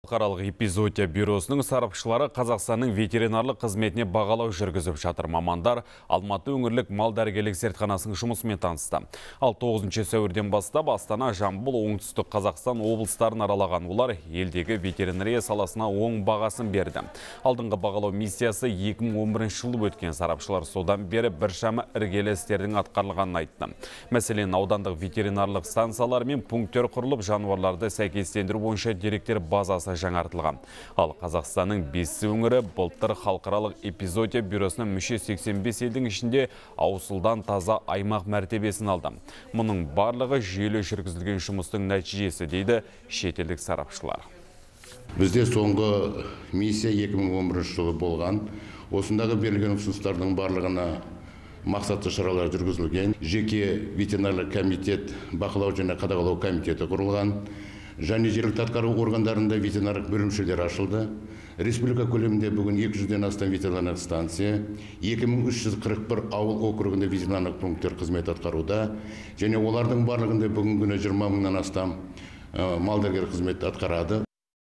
Сарапшылар еліміздің ветеринария қызметін бағалады
Халықаралық эпизоотия бюросының сарапшылары Қазақстанның ветеринарлық қызметіне бағалау жүргізіп жатыр. Мамандар Алматы өңірлік мал дәрігерлік зертханасының жұмысымен танысты. Ал, 9-сәуірден бастап Астана, Жамбыл, Оңтүстік Қазақстан облыстарын аралаған олар елдегі ветеринария саласына оң бағасын берді. Өйткені, алдыңғы бағалау миссиясы 2011 жылы өткен. Сарапшылар содан бері біршама іргелі істердің атқарылғандығын айтты. Мәселен, аудандық ветеринарлық станциялар мен пунктер құрылып, жануарларды сәйкестендіру бойынша деректер базасы жаңартылған. Ал, Қазақстанның 5 өңірі былтыр халықаралық эпизоотия бюросына мүше 85 елдің ішінде аусылдан таза аймақ мәртебесін алған. Мұның барлығы «Жүйелі жүргізілген жұмыстың нәтижесі», - дейді шетелдік сарапшылар.